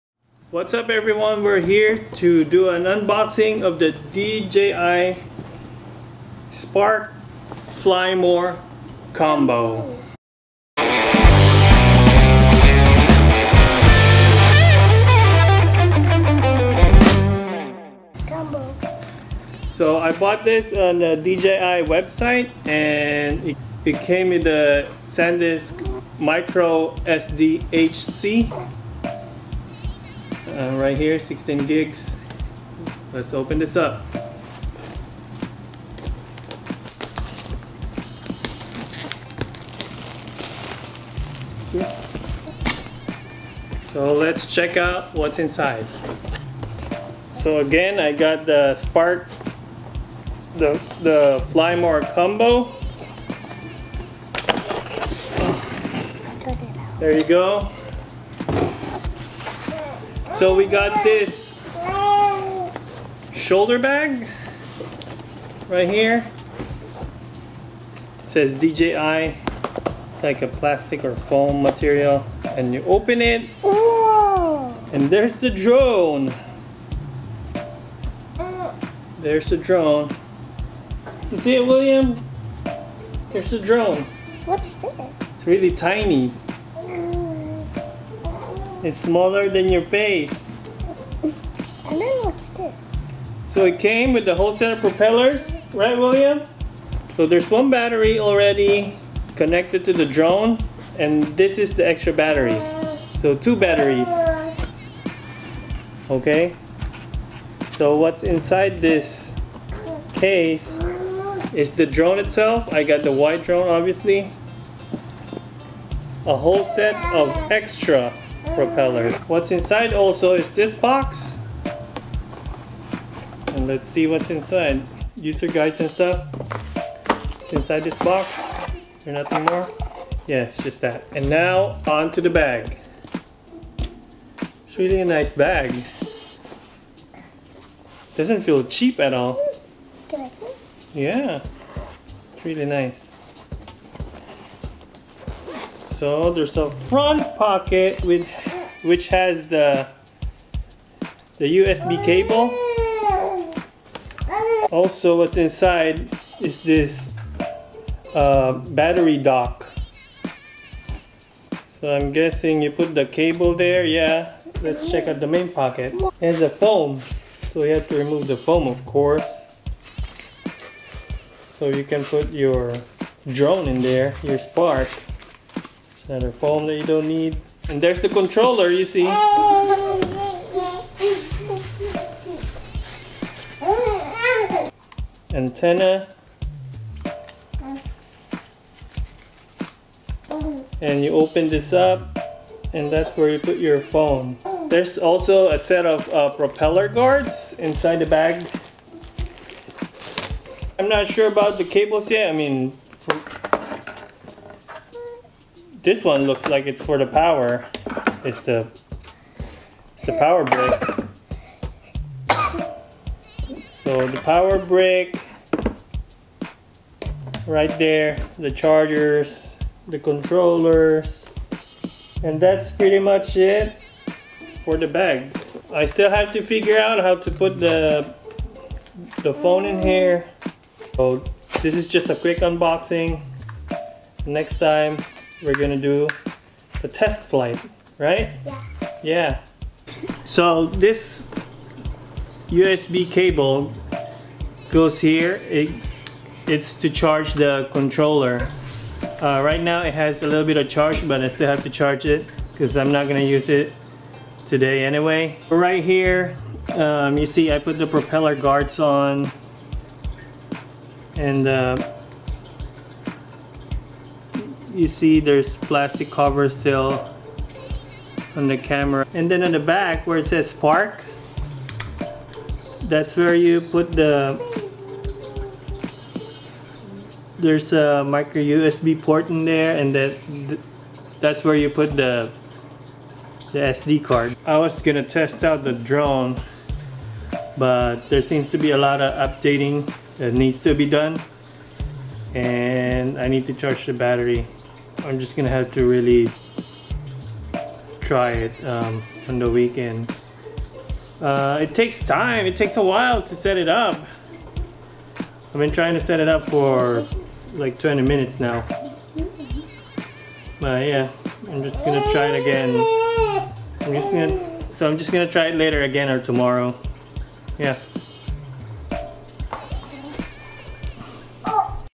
DJI Unboxing
Quick unboxing of the DJI Spark Drone.
Sorry for the OOF at the last part video.